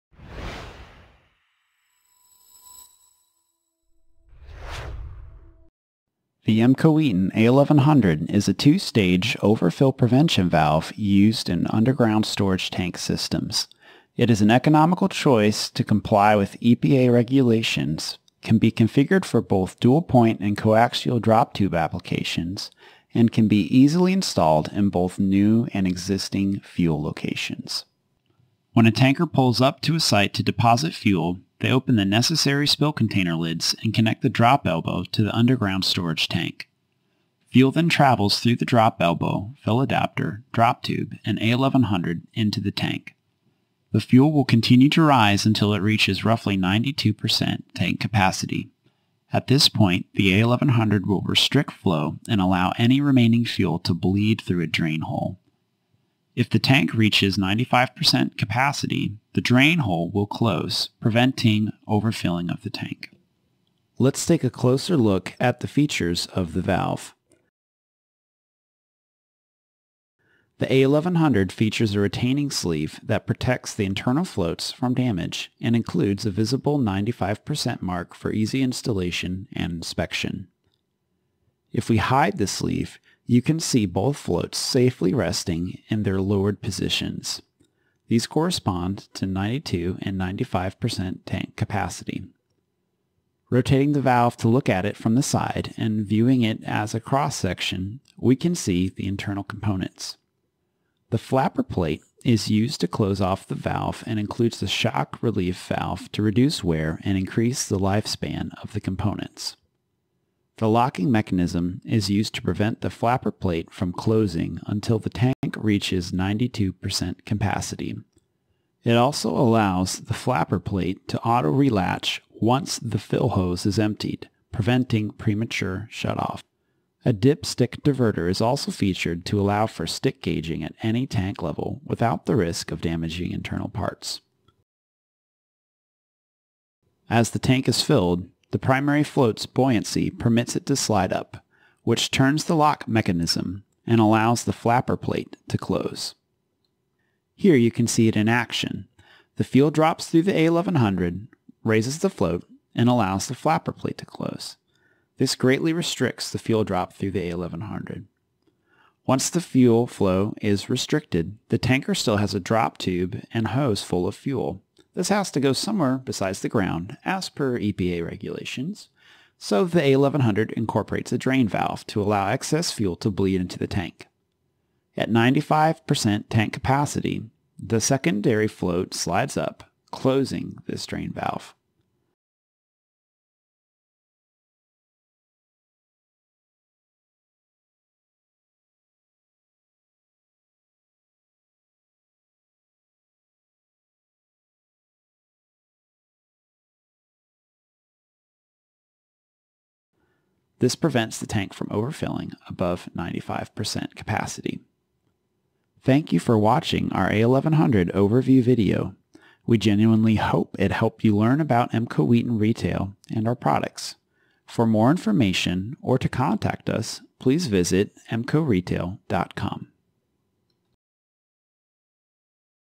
A1100 Overfill Prevention Valve Functional Animation
This video demonstrates the function and features of the Emco Wheaton Retail A1100 Overfill Prevention Valve.